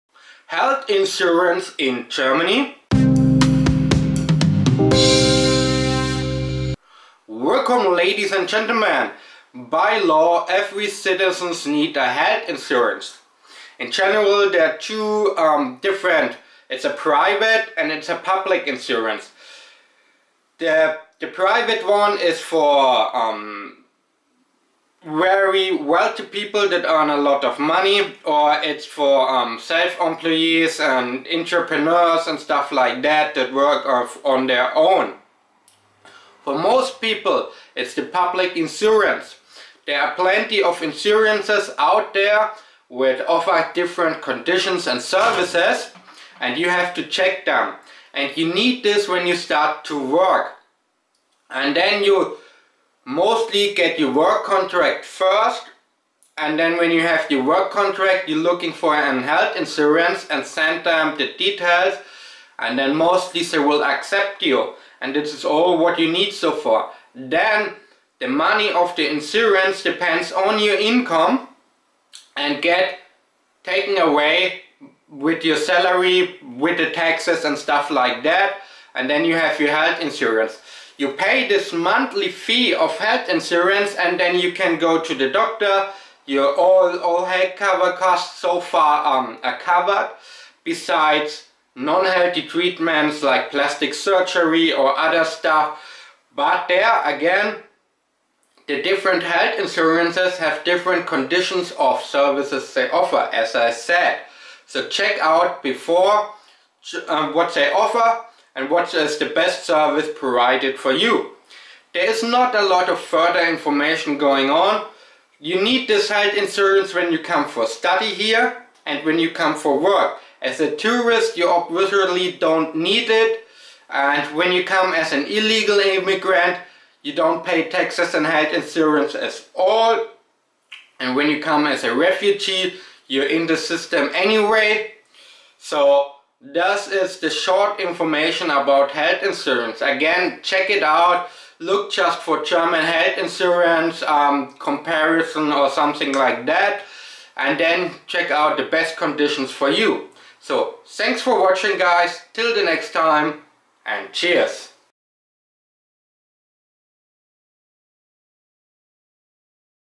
Germany, how it is: Healthcare in Germany?
Do you need a health insurance in Germany?

Living in Germany, Life in Germany